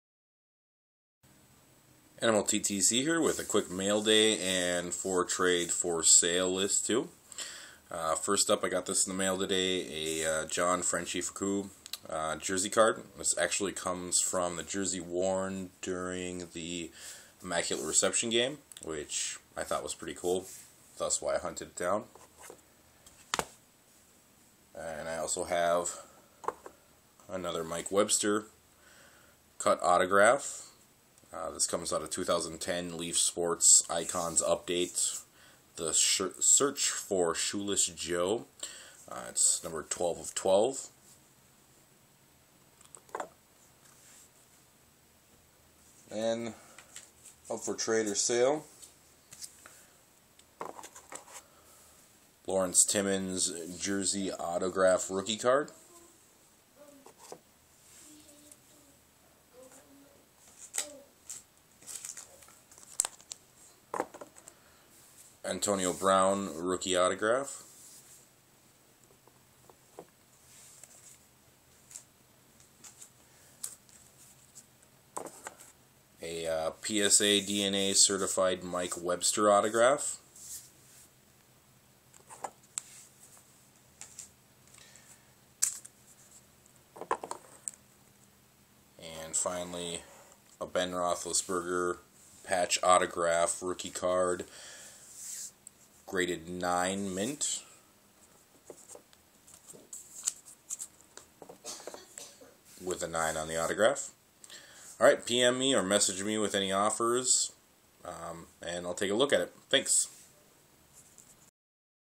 Mail Day 3-10-12 and some FT/FS cards
Cards For Trade/Sale:
07 Topps Triple Threads Lawrence Timmons Rookie Auto Jersey #104, 02/99
10 Panini Gridiron Gear Antonio Brown Rookie Auto #155, 139/199
83 Topps Mike Webster PSA/DNA certified auto
04 Hot Prospects Ben Roethlisberger Rookie Auto Jersey #86, 099/150 GRADED 9 w/ 9 auto

PM or Comment with any offers. On Trades only Steelers cards please.